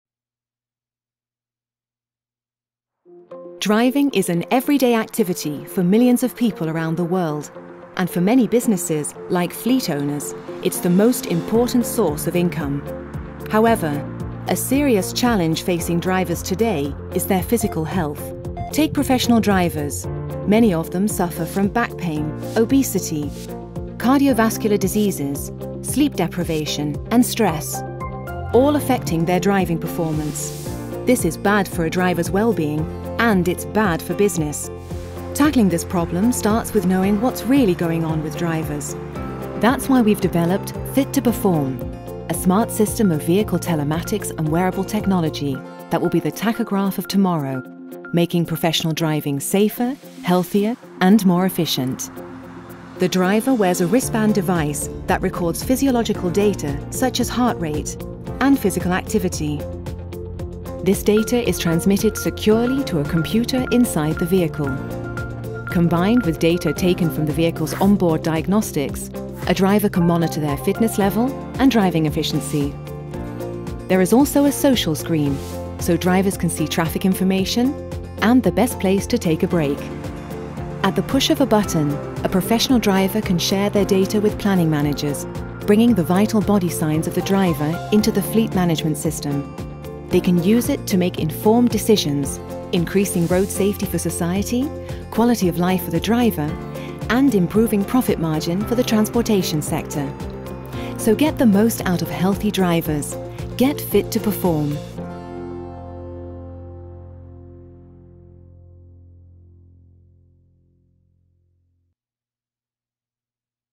EIT Digital' Fit to Perform Tachograph of tomorrow
Fit 2 Perform will deliver the tachograph of tomorrow making professional driving safer, healthier and more efficient. It brings the vital body signs (like activity, stress, sleep, …) of the driver into the fleet management systems next to the vehicle operation data (like acceleration, breaking, steering, …) and the operating environment data (like weather condition, traffic density, traffic jam, …) to close the loop. Fit to Perform will create a triple win: 1: increased road safety for the society, 2: improved quality of life for the driver, 3: improved profit margin for the transportation sector.